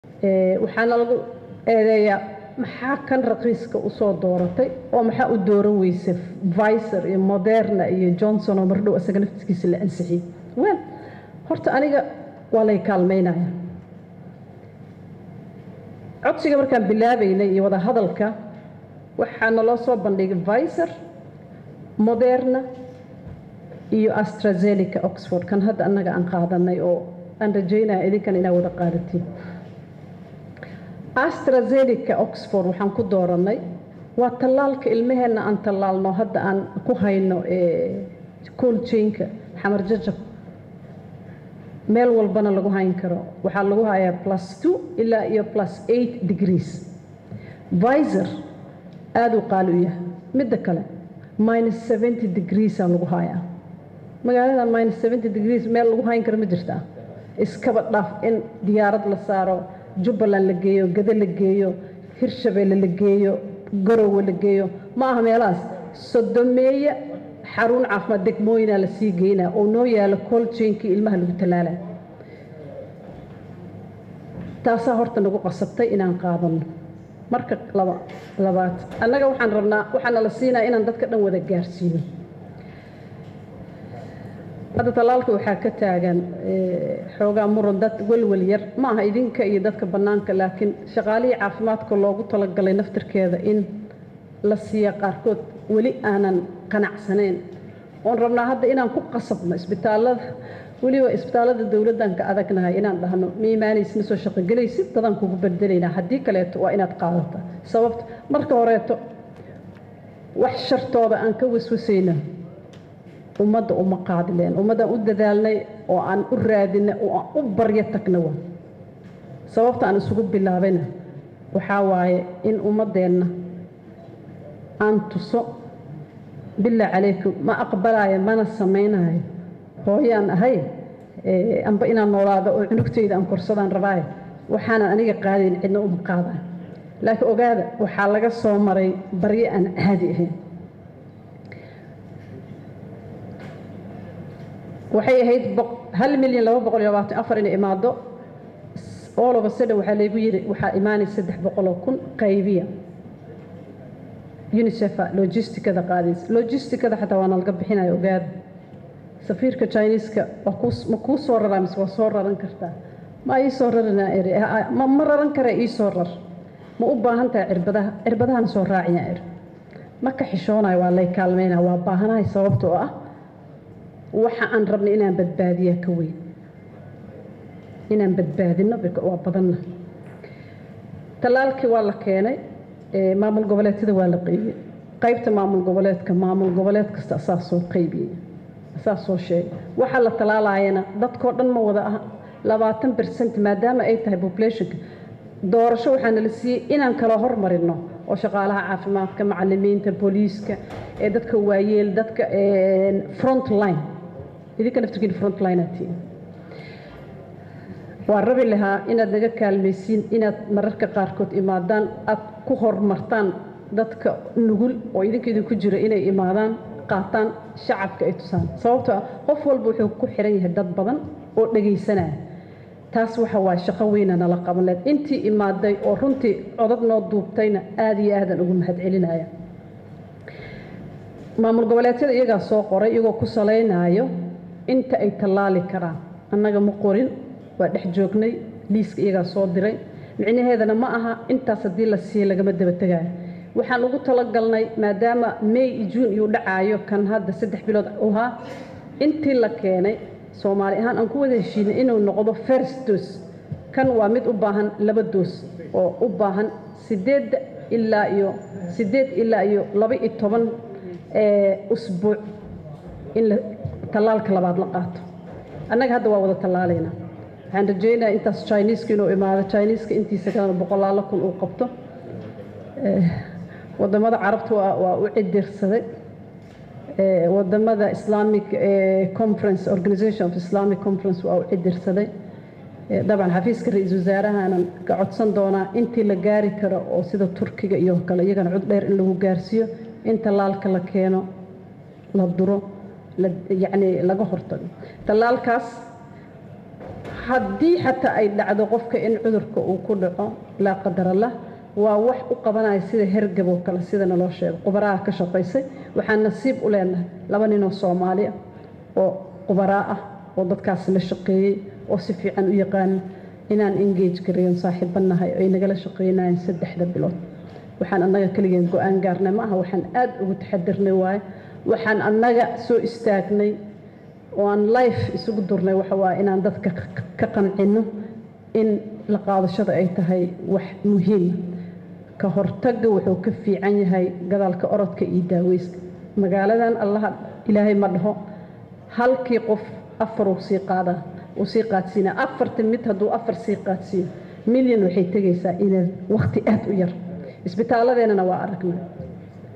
WASIIRKA CAAFIMAADKA OO KA HADASHAY XAALADDA COVID-19
Wasiir Fawziya Abiikar:  “Sababta aan isugu billaabay waxaa waaye in umaddeena aan tuso…”